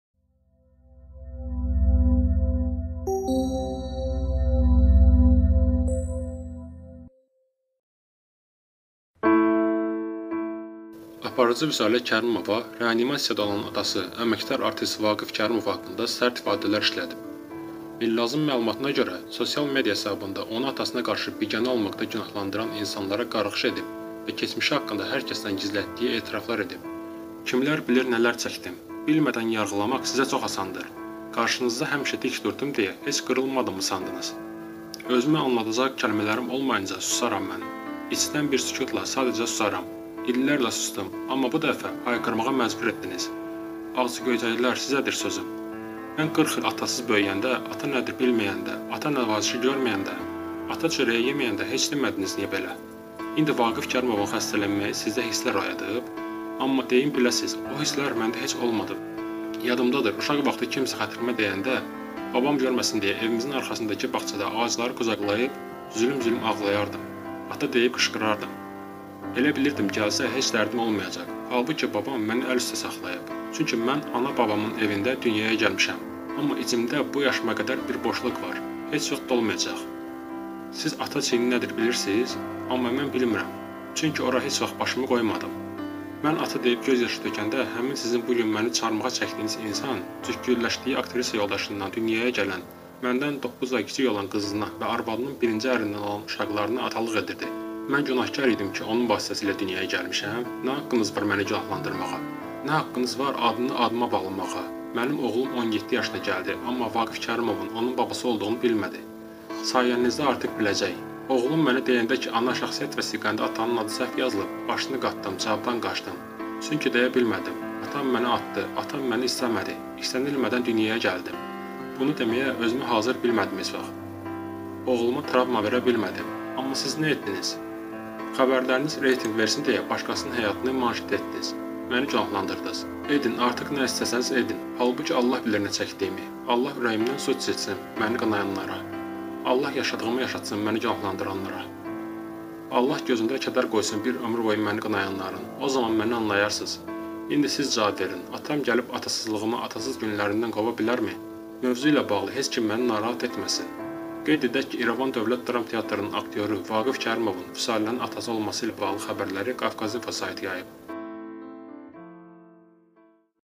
Vüsalə: "Mən ağlayanda, “cükküldəşdiyi” aktrisanın uşaqlarına baxırdı"
Aparıcı Vüsalə Kərimova reanimasiyada olan atası, əməkdar artist Vaqif Kərimov haqqında sərt ifadələr işlədib.

"Kimlər bilir, nələr çəkdim?! Bilmədən yarğılamaq sizə çox asandır! Qarşınızda həmişə dik durdum deyə, heç qırılmadımmı sandınız? Özümü anladacaq kəlmələrim olmayınca susaram mən... içdən, bir sükutla, sadəcə, susaram... İllərlə susdum! Amma bu dəfə məni hayqırmağa məcbur etdiniz! Ağzıgöyçəklər, sizədir sözüm!

Mən 40 il atasız böyüyəndə, ata nədir bilməyəndə, ata nəvazişi görməyəndə, ata çörəyi yeməyəndə heç demədiniz niyə belə? İndi Vaqif Kərimovun xəstələnməyi sizdə hisslər oyadıb? Amma deyim, biləsiniz, o hisslər məndə heç olmadı ki, bu gün də ölsün! Yadımdadır, uşaq vaxtı kimsə xətrimə dəyəndə, babam görməsin deyə evimizin arxasındakı bağçada ağacları qucaqlayıb, zülüm-zülüm ağlayırdım, ata deyib qışqırırdım...

Elə bilirdim, gəlsə, heç dərdim olmayacaq. Halbuki, babam məni əl üstə saxlayıb. Çünki mən ana babamın evində dünyaya gəlmişəm. Amma içimdə bu yaşıma qədər bir boşluq var, heç vaxt dolmayacaq. Siz ata çiyni nədir bilirsiniz? Amma mən bilmirəm, çünki ora heç vaxt başımı qoymadım!

Mən ata deyib, göz yaşı tökəndə həmin sizin bu gün məni çarmıxa çəkdiyiniz insan "cükküldəşdiyi" aktrisa yoldaşından dünyaya gələn, məndən doqquz ay kiçik qızına və arvadının birinci ərindən olan uşaqlarına atalıq edirdi!

Mən günahkar idim ki, onun vasitəsilə dünyaya gəlmişəm? Nə haqqınız var məni günahlandırmağa? Nə haqqınız var adını adıma bağlamağa? Mənim oğlum 17 yaşına gəldi, amma Vaqif Kərimovun onun babası olduğunu bilmədi!

Sayənizdə artıq biləcək! Oğlum mənə deyəndə ki, ana şəxsiyyət vəsiqəndə atanın adı səhv yazılıb, başını qatdım, cavabdan qaçdım. Çünki deyə bilmədim, atam məni atdı, atam məni istəmədi, istənilmədən dünyaya gəldim... Bunu deməyə özümü hazır bilmədim heç vaxt, oğluma travma verə bilmədim... Amma siz nə etdiniz?

Xəbərləriniz reytinq versin deyə başqasının həyatını manşet etdiniz! Məni günahlandırdınız! Edin! Artıq nə istəsəniz, edin! Halbuki Allah bilir nə çəkdiyimi! Allah ürəyimdən su içirtsin məni qınayanlara! Allah yaşadığımı yaşatsın məni günahlandıranlara!